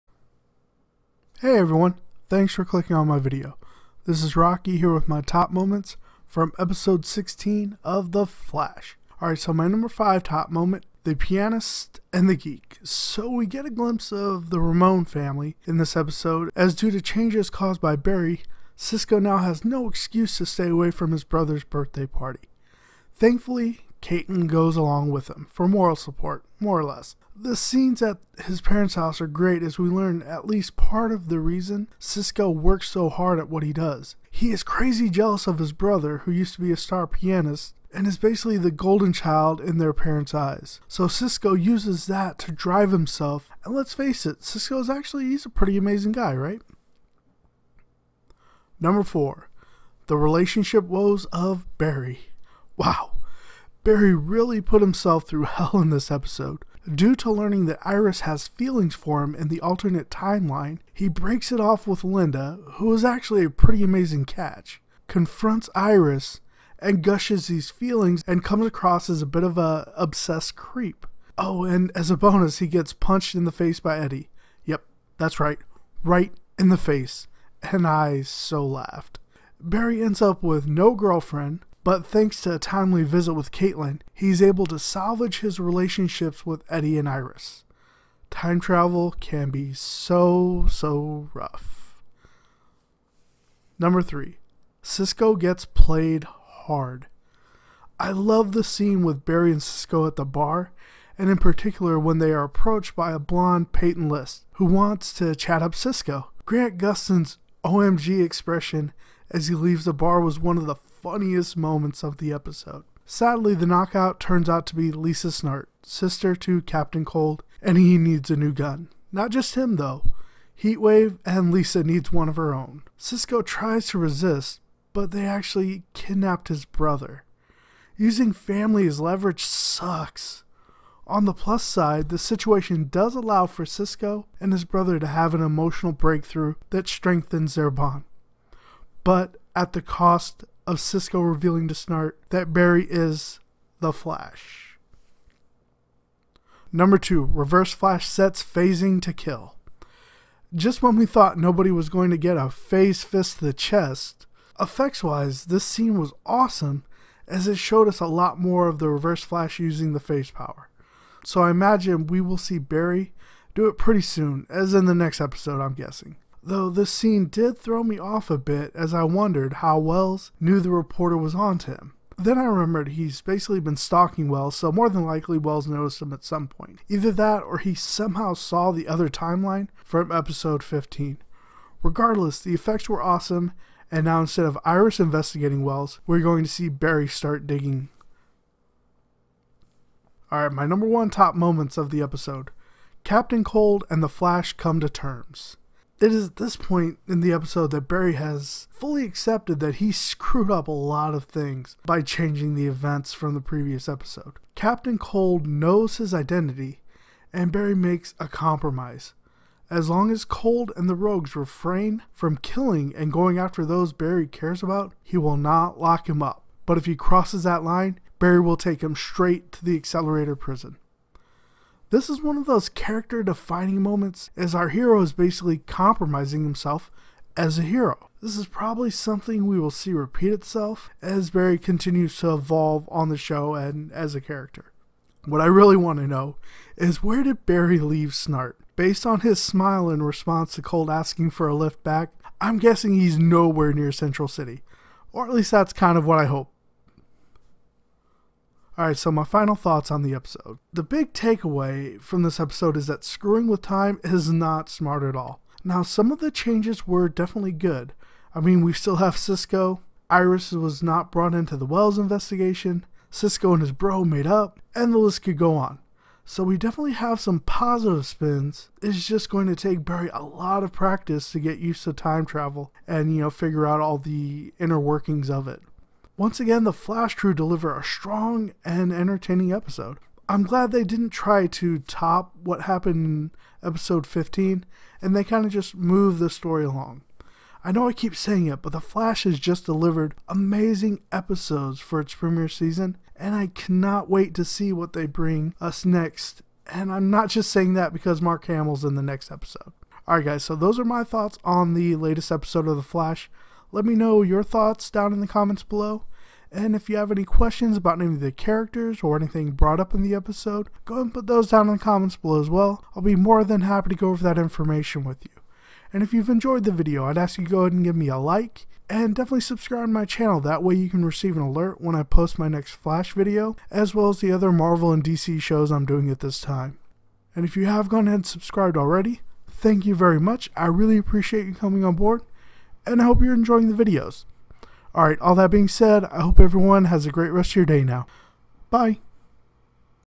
Top Moments The Flash Season 1 Episode 16 Rogue Time
#5 The pianist and the geek
So we get a glimpse of the Ramone family in this episode.  As due to changes caused by Barry Cisco now has no excuse to stay away from his brothers birthday party.  Thankfully Caitlin goes with him.  The scense at his parents house are great as we learn at least part of the reason Cisco works so hard.  He is crazy jealous of his brother who used to be a star pianist and is basically the golden child in their parents eyes.  So Cisco uses that to drive himself and lets face it he is pretty amazing.

#4 The relationship woes of Barry
Wow Barry really put himself through hell in this episode.  Due to learning that Iris has feelings for him in the alternate timeline he breaks it off with Linda...who was actually a pretty amazing catch.  Confronts Iris and gushes these feelings and comes across as a bit of a obsessed creep.  Oh and as a bonus he gets punched in the face by Eddie...yep right in the face and I laughed.  Barry ends up with no girlfriend but thanks to a timely visit from Caitlin he is able to salvage his realtionships with Eddie and Iris.  Time travel can be rough.

#3 Cisco gets played hard
I loved the scene with Barry and Cisco at the bar and in particular when they are approached by a blonde Peyton List who wants to chat up Cisco.  Grant Custin's OMG expression as he leaves the bar was one of the funniest moments of the night.  Sadly the kncockout turns out to be Lisa Snart sister to Captain Cold and he needs a new gun.  Also Heatwave and Lisa do as well.  Cisco tries to resist but they kidnapped his brother.  Using family as leverage sucks... On the plus side the situation does allow for Cisco and his brother to have an emotional breakthrough that strengthens their bond...but at the cost of Cisco revealing to Snart that Barry is The Flash.

#2 Reverse Flash seats phasing to kill
Just when we thought nobody was going to get a pahsed fist to the chest...So effect sie this scene was awesome as it showed us a lot more of the reverse flash using the phase power.  So I imagine we will see Barry do it pretty soon as in the next episode.  Though this scene did throw me off a bit as I wondered how Wells knew the reporter was onto him.  Then I remembered he has basically been stalking Wells...so that is preobably the case.  Either that or he somehow saw the other timeline from episode 15.  Regardless the effects were awesome and now instead of Iris investigating wells we are going to see Barry start digging.

#1 Captain Cold and The Flash come to terms
It is at this point that Barry had fully accepted that he screwed up a lot of things but changing events. Captain Cold know his identity and Barry makes a compromise.  As long as Cold and the Rogues refrain from killing and going after those Barry cares about he will not lock him up but if he crosses that line Barry will take him straight to the accelorator prison.  This is one of those character defining moments as our hero is basically comprmising himself as a hero.  This is probably something we will see repeat itself as Barry evolves.  What I really want to know is where did Barry leave Snart.  Based on his smile in response to Cold asking about a lift back I am guessing nowhere near Central City.

Final Thoughts
So our big take away from this episode is that screwing with time is not smart at all.  Now some of the changes were definitely good I mean we still have Cisco, Iris was not brought into the Wells investigation, Cisco and his bro made up and the list could go on.  So we definitely have some positive spins it is just going to take Barry a lot of practice to get time travel figured out.  Once again The Flash crew deliver a strong and entertaining episode.  I am glad they did not try and top what happened in episode 15 and just moved the story along.  I know I keep saying it but The Flash has just delivered amazing episodes for its premier season and I cannot wait to see what they bring us next and I am not just saying that since Mark Hamill is in the next episode.

You can find me via the following socials.